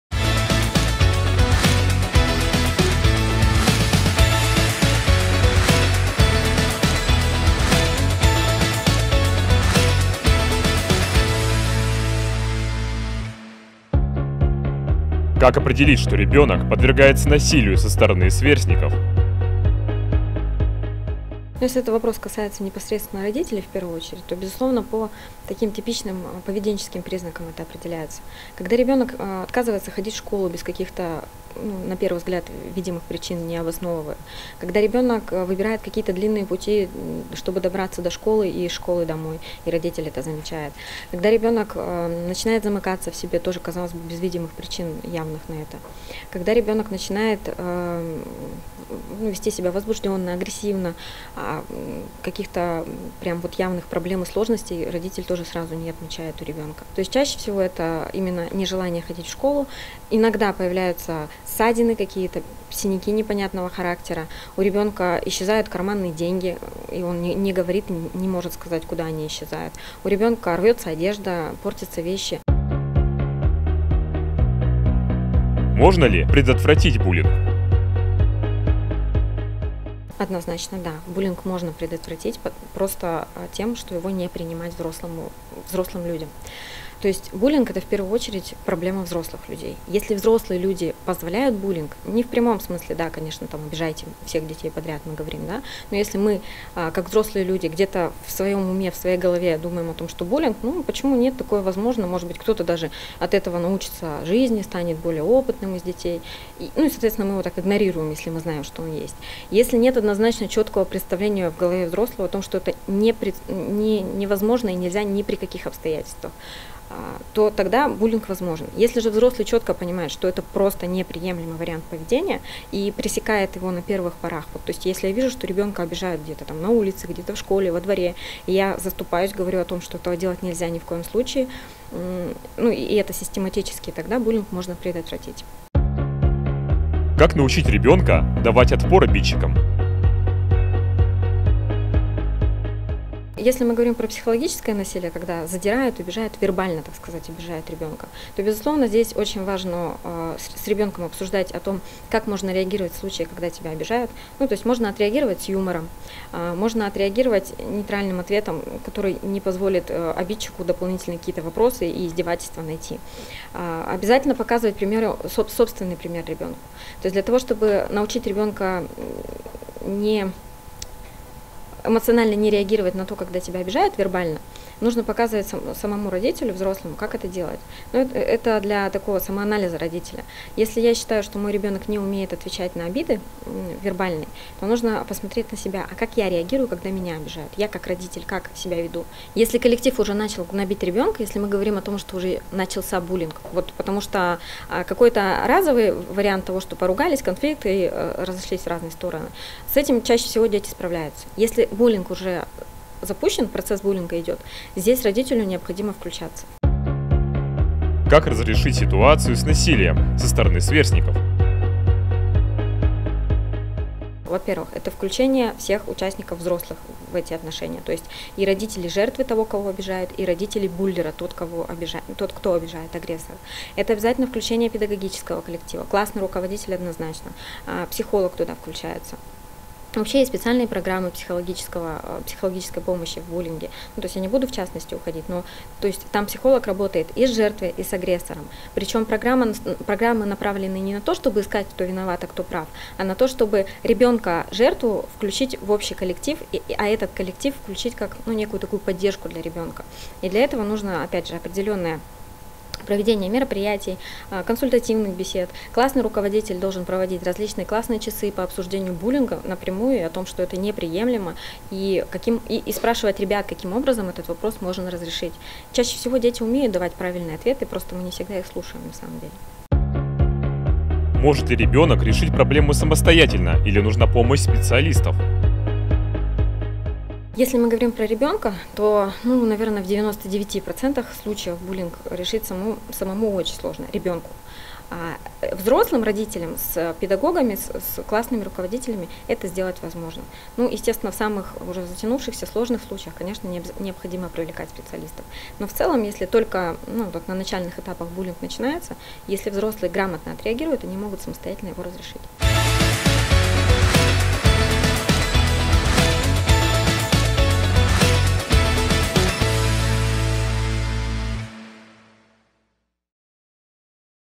5 вопросов о буллинге
Конфликты в школе или на улице  - явление неизбежное. Подростки учатся отстаивать и защищать свою точку зрения и принимать чужую. Совсем другое, когда конфликты становятся систематическими и перерастают в травлю. Как выявить буллинг и как ему противостоять, на 5 вопросов отвечает педагог-психолог центра «Семья»  Дарья Петина.